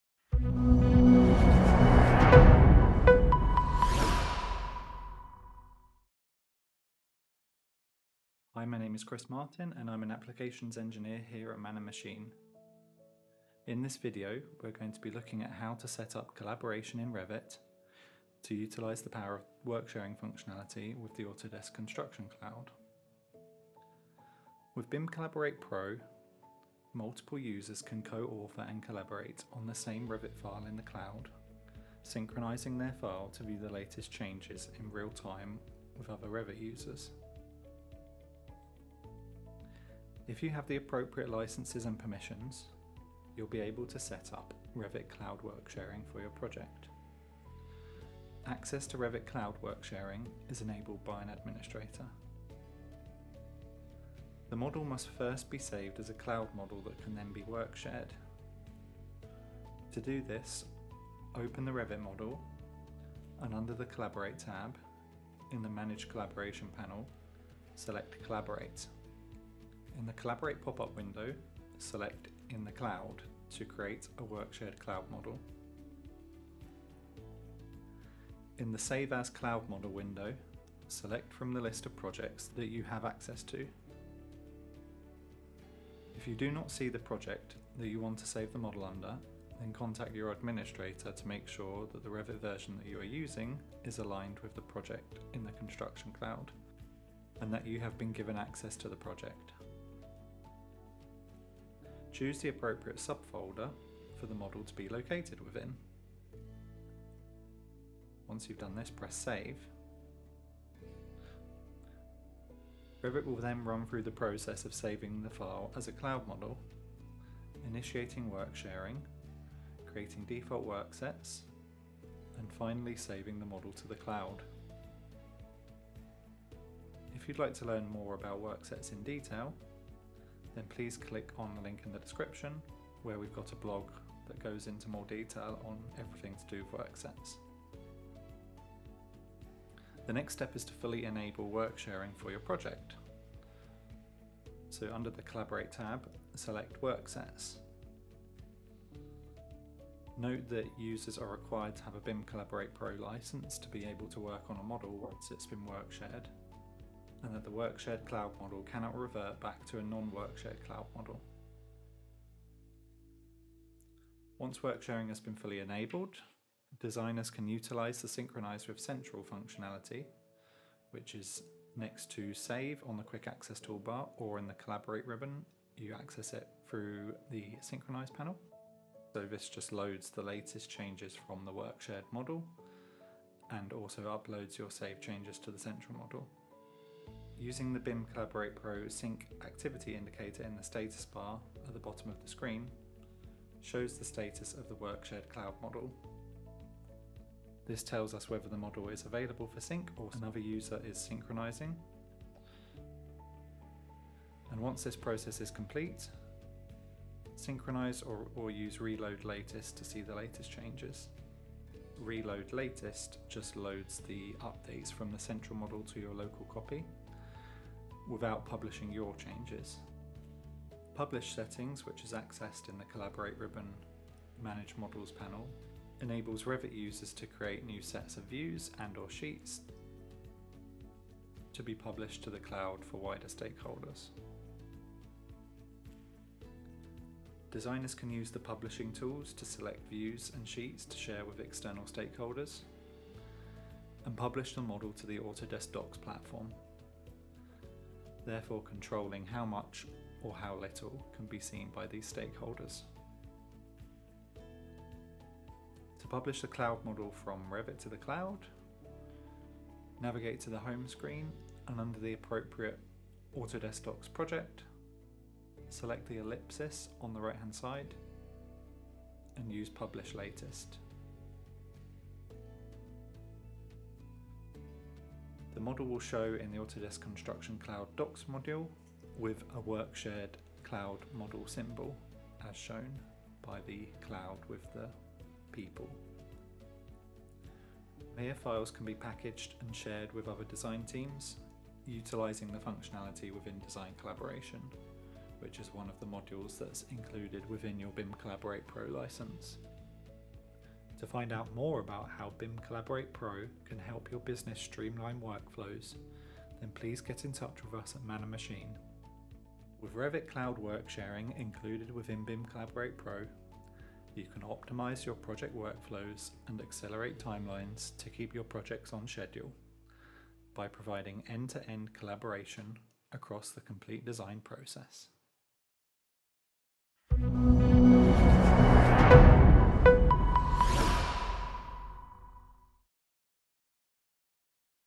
How To Set Up Revit Cloud Worksharing with BIM Collaborate Pro
In this video, we will be demonstrating the process of setting up Cloud Worksharing for your Revit model. This feature allows for multi-user, real-time collaboration in Revit facilitated by BIM Collaborate Pro and the Autodesk Construction Cloud (ACC). By co-authoring, centralising project data in the cloud, and utilising the wider functionality of ACC, you and your teams can streamline workflows and accelerate project timelines.